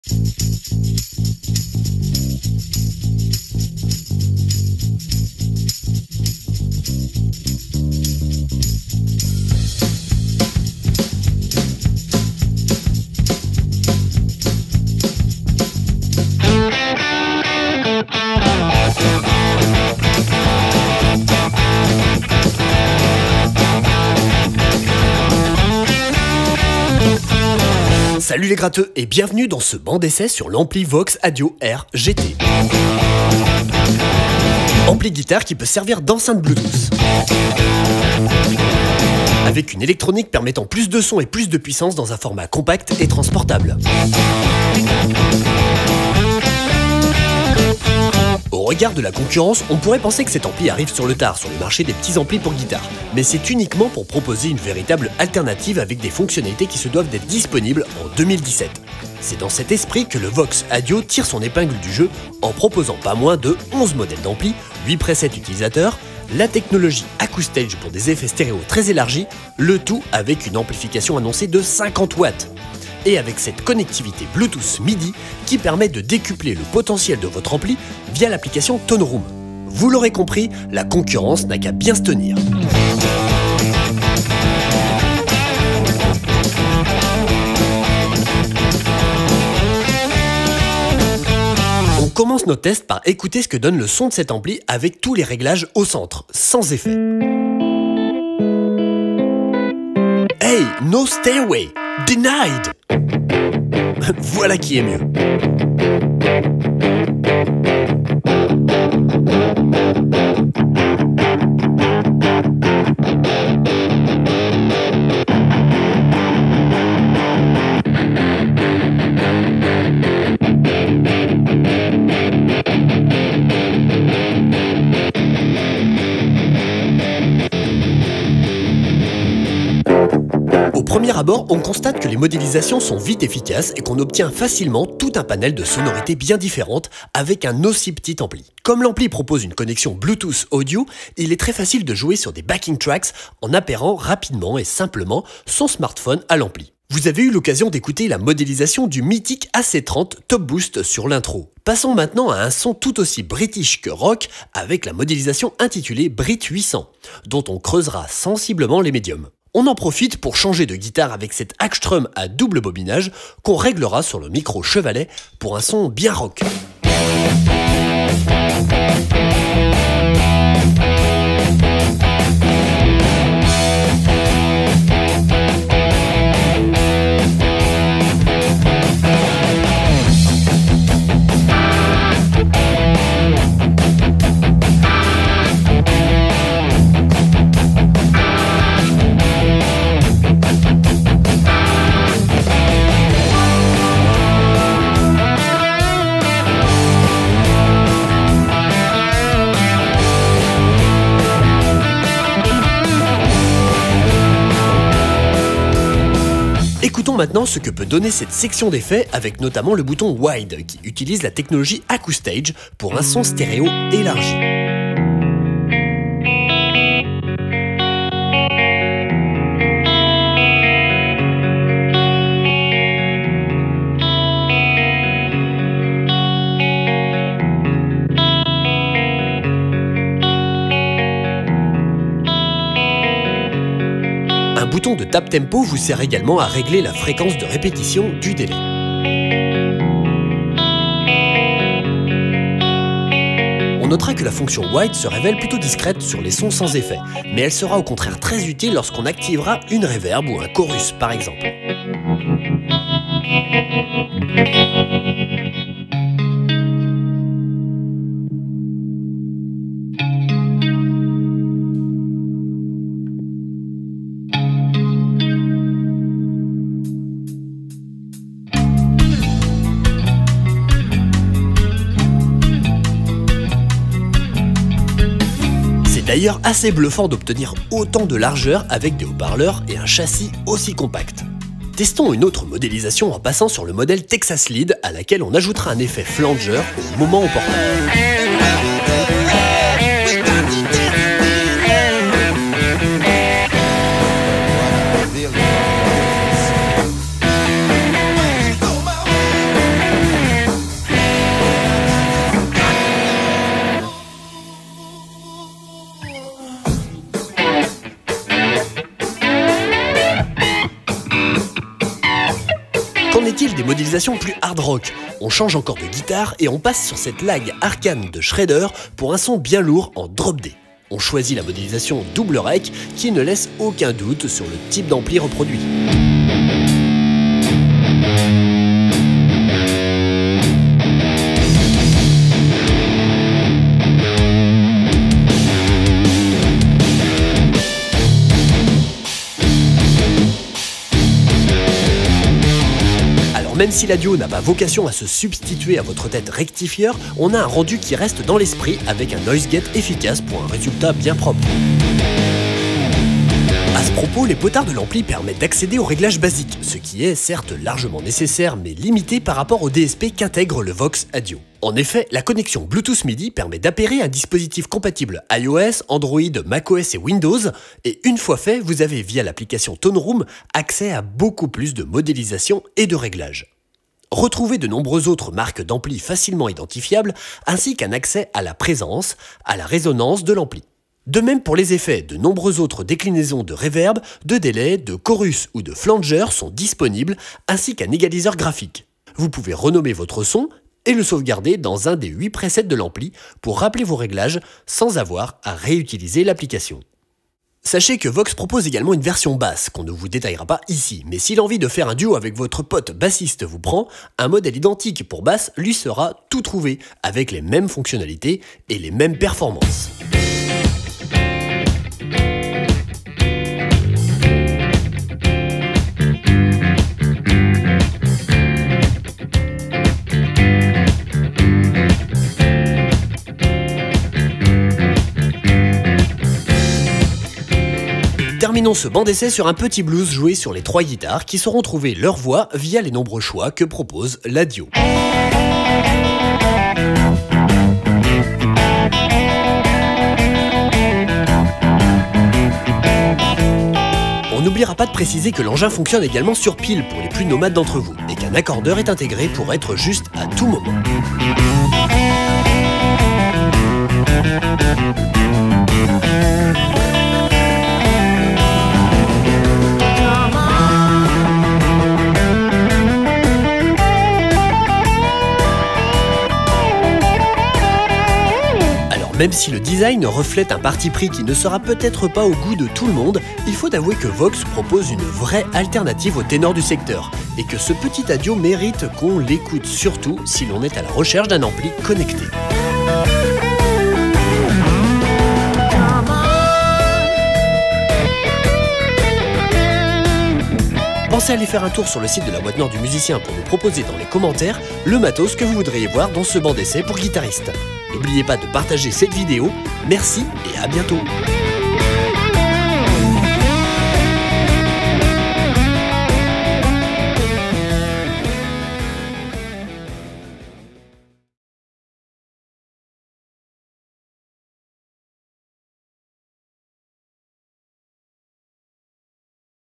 Banc d'essai ampli guitare VOX ADIO air GT et enceinte bluetooth (vidéo de La Boite Noire)
Visionnez un banc d'essai complet sur l'ampli guitare et enceinte Bluetooth VOX ADIO.

L'ampli guitare VOX ADIO AIR GT propose de nombreuses modélisations d'ampli et des fonctionnalités dépassant la concurrence comme la connectivité Bluetooth et l'interface avec l'application Tone Room pour un contrôle total de l'ampli.

FICHE TECHNIQUE
- Modèles d’amplis : 11 (23 accessibles via le logiciel Tone Room)
- Effets 1 (modulation) : 4 (11 accessibles via le logiciel Tone Room)
- Effets 1 (délai/réverbe) : 4 (8 accessibles via le logiciel Tone Room)
- Programmes : 8 (2 banques de 4 patches)
- Effet Wide Stereo (Virtual Surround) : on/off
- Réducteur de bruit : off, 1 - 4 niveaux
- Égaliseur :off, loud, bass boost, mid boost, treble boost
- Entrées/Sorties : entrée jack, entrée jack Aux, entrée casque jack, port USB (type B)
- Sans fil : Bluetooth & Bluetooth low energy (Midi)
- Amplification : maximum approx. 25 W x 2 RMS @ 8 ohms (avec piles : maximum approx. 2,5 W x 2 @ 8 ohms)
- h.p. : 2 x 3", 8 ohms
- Inclus : bloc d’alimentation + câble, code JamVox III
- Alimentation : 8 piles AA (8 heures avec des alcalines) ou l’adaptateur secteur AC (DC 19 V) inclus
- Consommation électrique : 3,42 A (sur piles 200 mA)
- Dimensions : 360 x 165 x 163 mm
- Poids : 2,9 kg

Modèles d’amplis : Deluxe CL (normal), AC30, Boutique CL, AC30TB, Boutique OD, Texas Lead, Brit 1959 (normal), Brit 800, Brit VERB, Double Rec, Flat
Avec Tone Room : Deluxe CL vibrato, Tweed 4 x 10 bright, Tweed 4 x 10, Brit 1959 treble, Brit VM, SL-OD, Cali Elation, Erupt III CH2, Erupt III CH3, Boutique métal, Original CL, Acoustic simulator

Effets 1 : chorus (Stereo Chorus), flanger (Black Flanger), phaser (Org Phaser 1), trémolo (Twin Trem)
Avec Tone Room : CE Chorus, STEREO Flanger, Org Phaser 2, Small Phase, Duo Phase (Free), Duo Phase (Sync), Harmonic Trem

Effets 2 : Analog Delay, Wide Delay, Spring, Hall
Avec Tone Room : Tape, Cross Delay, Room, Plate